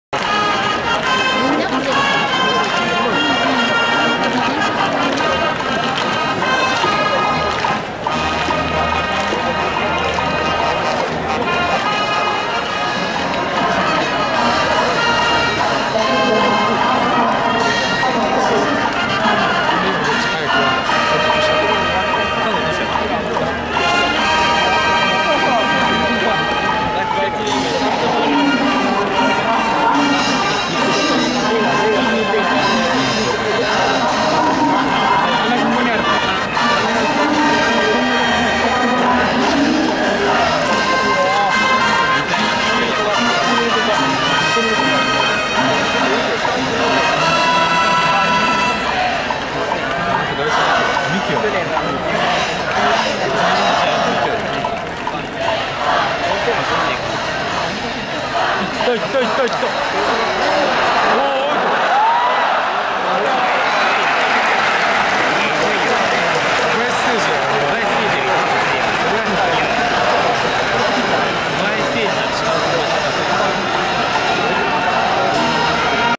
高校日本代表捕手 山瀬選手（星稜）の強肩
代走・田中幹也外野手（亜細亜大学）の二盗を
山瀬慎之助捕手（星稜）が阻止してピンチを切り抜けた。